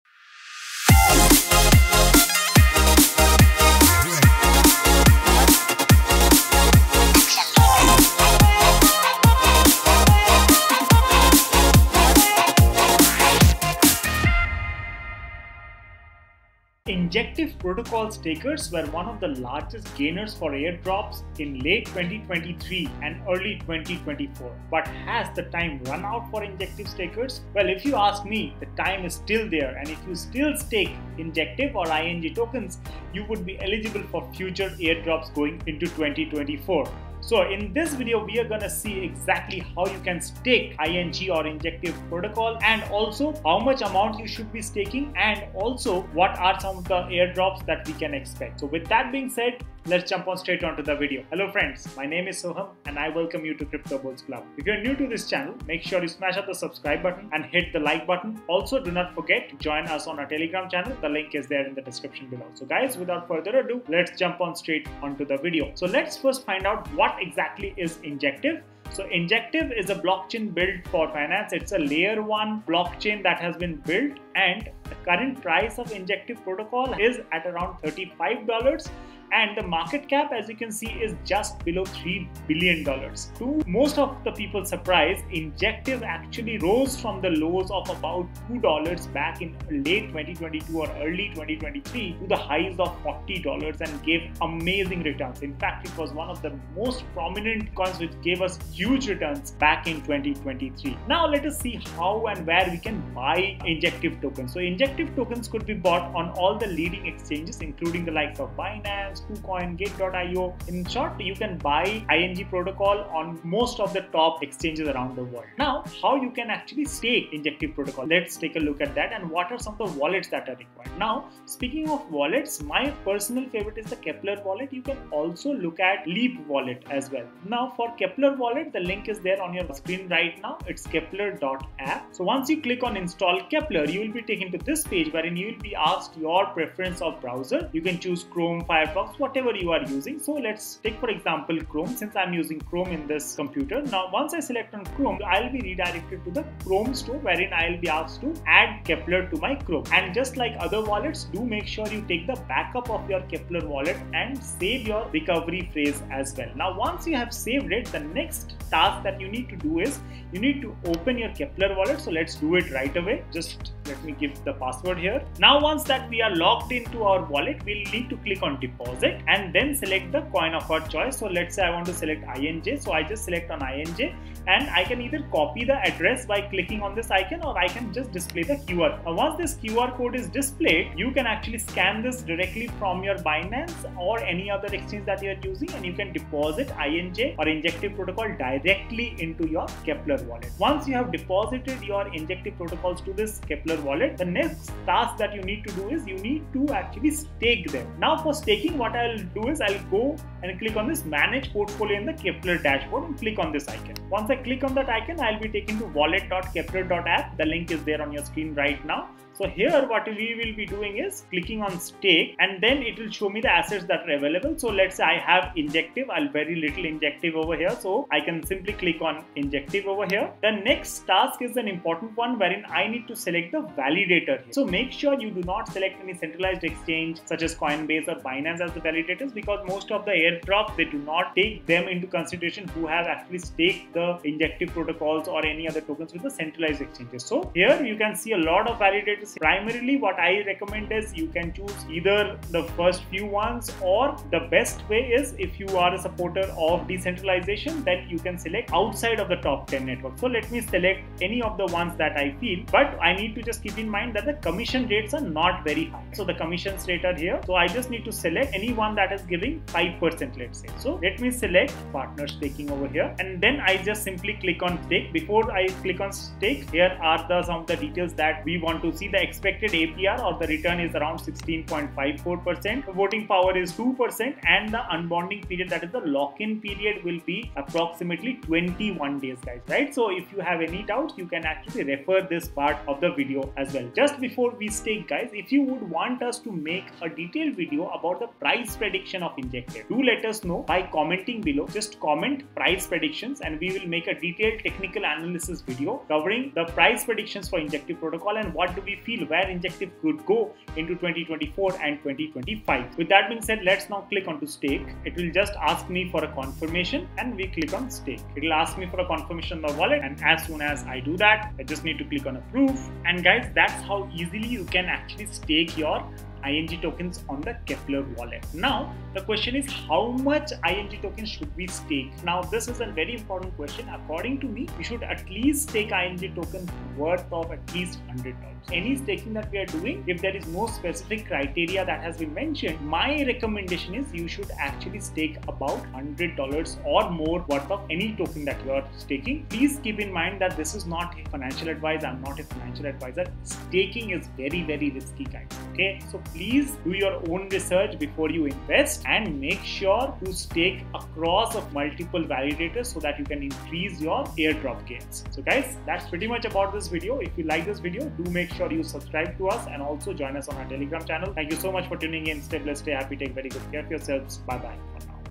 A Comprehensive Guide to Staking Injective Protocol (INJ) | Airdrops Coming | Crypto Airdrops 2024
Description:
🚀 Ready to supercharge your crypto earnings? Dive into our in-depth tutorial on staking Injective Protocol's (INJ) and unlock the potential for exclusive airdrops! 💸 Whether you're a seasoned investor or a crypto enthusiast, this step-by-step guide covers everything you need to know about staking INJ and maximizing your rewards.

🔒 Learn the ins and outs of staking, understand the eligibility criteria for airdrops, and discover the optimal staking amounts for maximizing your returns. Our detailed walkthrough ensures that you'll navigate the staking process with ease, allowing you to harness the full power of Injective Protocol.

🌐 From setting up your wallet to choosing the right staking pool, we've got you covered. Watch the video now to become a staking pro and position yourself for upcoming airdrops that could significantly boost your crypto portfolio.

🎓 What's Inside:

Setting up your wallet for INJ staking
Choosing the right staking pool for maximum rewards
Understanding eligibility criteria for exclusive airdrops
Optimizing staking amounts for higher returns
Pro tips and strategies to enhance your staking experience
💡 Don't miss out on this opportunity to elevate your crypto game! Hit play now and embark on your journey to mastering Injective Protocol staking for lucrative airdrops. 🚀💰

Timestamp
00:00 - Introduction
01:05 - What is Injective (INJ)
01:15 - INJ Coin Price
01:45 - Where to buy INJ Coin
02:01 - Wallets Supporting INJ Coin
02:11 - How to Install Keplr Wallet
02:45 - How to Deposit INJ Coin to Keplr Wallet
03:10 - Step-by-Step Process on How to Stake INJ Coin
05:20 - How much INJ Coin should we stake?
05:41 - Outro

Do Your Own Research before investing